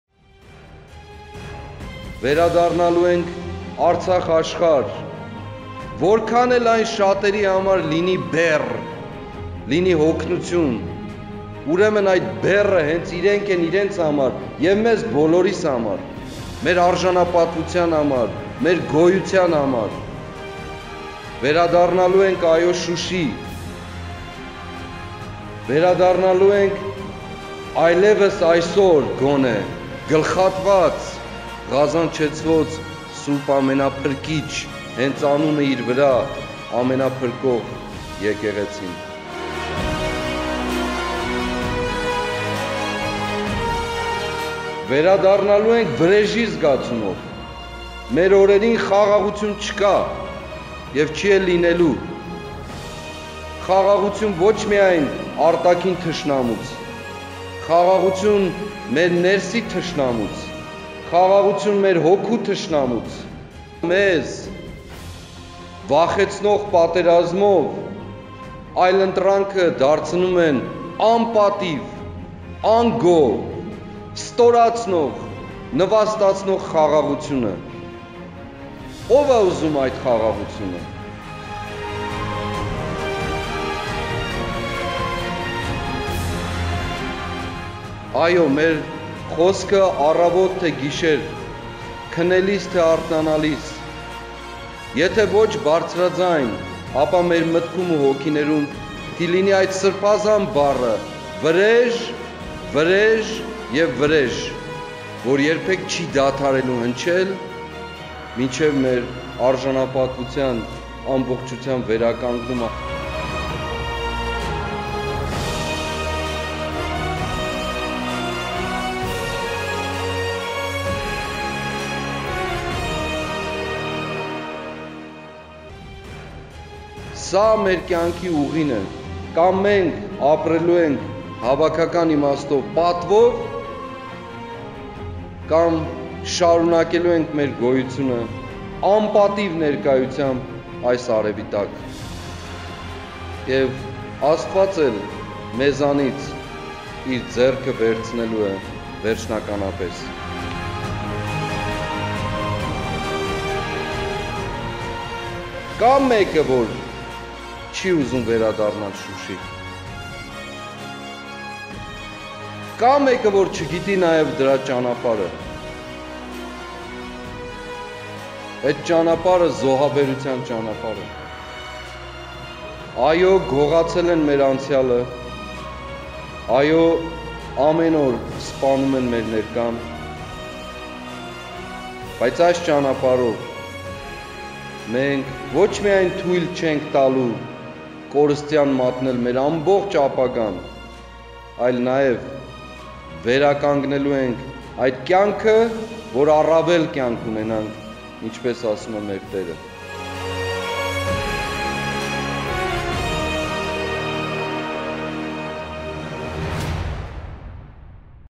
Շնորհավո՛ր տոնդ, ԱՆԿԱԽ ԱՐՑԱԽ
«Վերադառնալու ենք Արցախ աշխարհ՝ որքան էլ որ դա շատերի համար լինի բեռ, լինի հոգնություն։ Այդ բեռն իրենք են իրենց և մեզ բոլորիս համար։ Վերադառնալու ենք Շուշի, վերադառնալու ենք այսօր այլևս գլխատված Ղազանչեցոց Սուրբ Ամենափրկիչ։
Վերադառնալու ենք վրեժի զգացումով. մեր օրերին խաղաղություն չկա։
Գողացել են մեր անցյալը, ամեն օր սպանում են մեր ներկան, բայց ոչ միայն թույլ չենք տալու այս ճանապարհով կորստյան մատնել մեր ապագան, այլ նաև վերականգնելու ենք այդ կյանքը, որ առավել կյանք ունենանք, ինչպես ասում է մեր Տերը։
Կա՞ մեկը, որ չի ուզում վերադառնալ Շուշի, կա՞ մեկը, որ չգիտի դրա ճանապարհը։ Այդ ճանապարհը զոհաբերության ճանապարհն է»